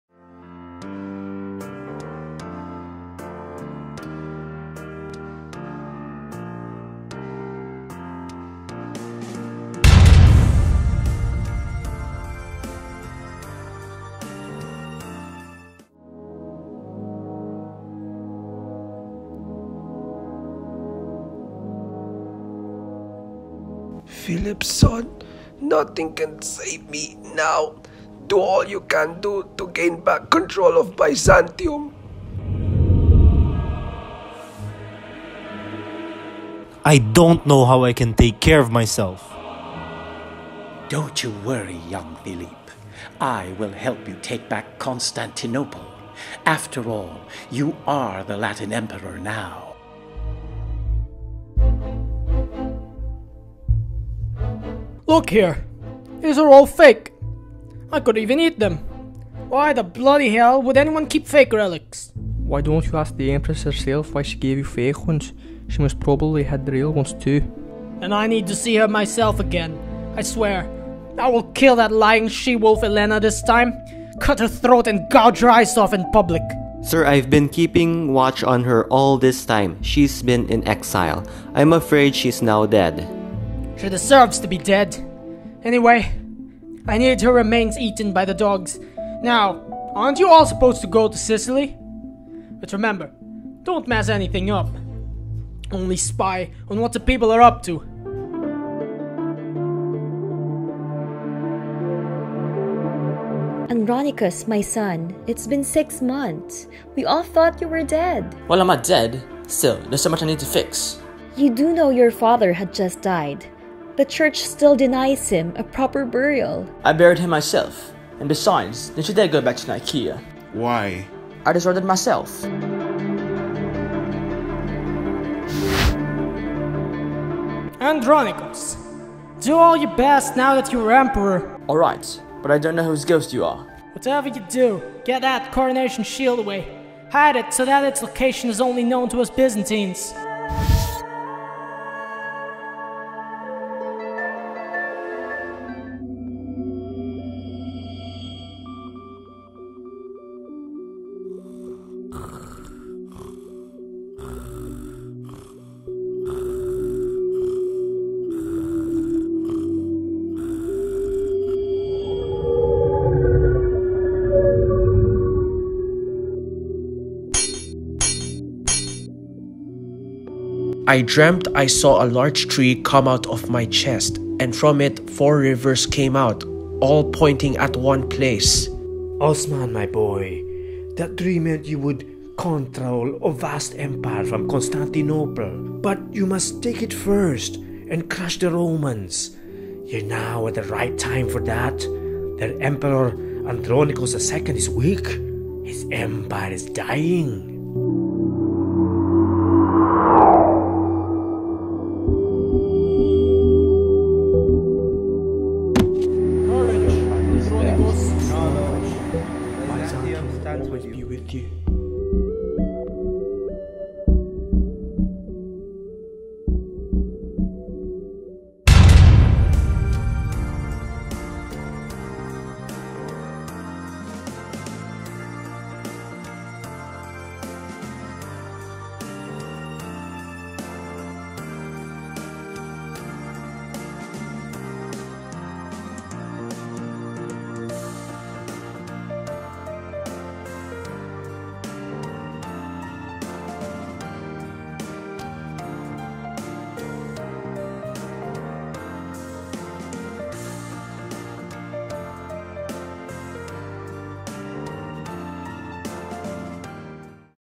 War of the Sicilian Vespers: The Untold Byzantine Epic (Deleted Scenes)
Warning, spoiler alert! Before you watch this, watch the main movie "War of the Sicilian Vespers: A Byzantine Epic" (2020) first:

Here is the director's cut one-shot film of the previous movie features 4 short story scenes not included in the movie but explains what has happened behind the story of the 1282 War of the Sicilian Vespers in Byzantium and in Western Europe. The 4 parts of this one-shot include:
Chapter 1- In 1273, the former ousted Latin emperor of Constantinople Baldwin II dies in Sicily leaving his only son Philippe Courtenay his heir under the protection of Sicily's French king Charles I of Anjou.
Chapter 2- Fast-forward to 1282, as Byzantine emperor Michael VIII Palaiologos briefs his men including his old time general Alexios Caesar before their first mission to Sicily (before the opening of the movie) he discovers the relics he took for himself in Nicaea back in 1261 are fake.
Chapter 3- At the end of 1282, Emperor Michael VIII Palaiologos of Byzantium had died travelling, meanwhile his son Andronikos returns to Constantinople after disappearing for 6 months to ascend to the imperial throne shocking his mother Theodora who had thought he died. While away, Andronikos out of frustration destroyed Nicaea, the former capital of the exiled Byzantium, however to his surprise he gets advice from the ghost of an old Byzantine emperor.
Chapter 4- Fast-forward to 1299, while Andronikos II turns out to be a weak emperor, Byzantine territory collapses in Asia Minor allowing a Turkish to conquer Byzantium. This Turkish warlord Osman Gazi dreams a mysterious dream of a tree which means he destined to destroy Byzantine civilization and found a new empire.

Directed by Powee Celdran
Voice Cast: Powee Celdran, Mario Puyat, Alina R. Co, Santiago Roxas, Igi Rollan, Angelo Lacson, JC Santos, Caleb Acorda, Cedric Parra, Carlos Francisco, with Andre Martin, and RJ Celdran
Sets by Lego